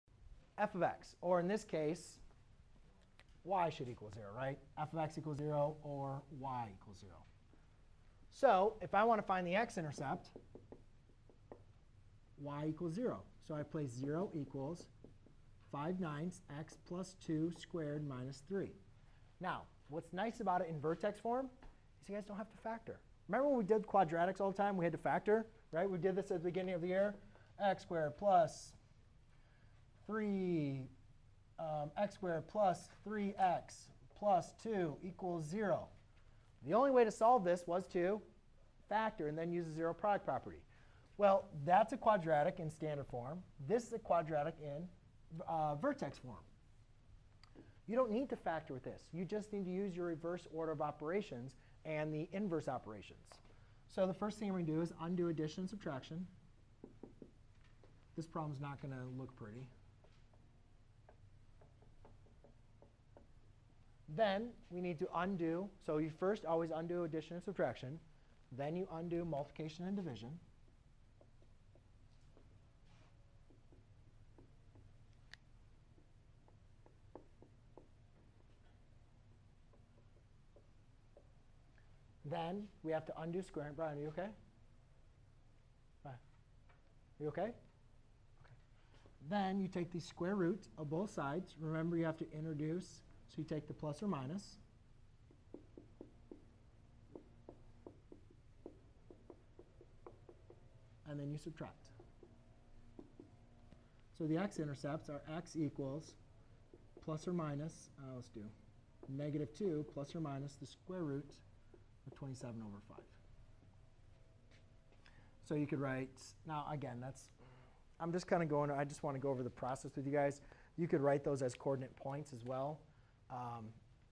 How to find the x intercepts (solutions) of a quadratic in vertex form
👉Learn how to solve quadratic equations using the square root method.  It is important to understand that not all quadratics have to be solved using factoring or quadratic formula.  When we only have one variable but it is squared we can just use inverse operations to isolate the variable.  The inverse operation of squaring is taking the square root.  Hence why we call this the square root method. All equations can be solved using the square root method but when there is more than one variable we have to complete the square first.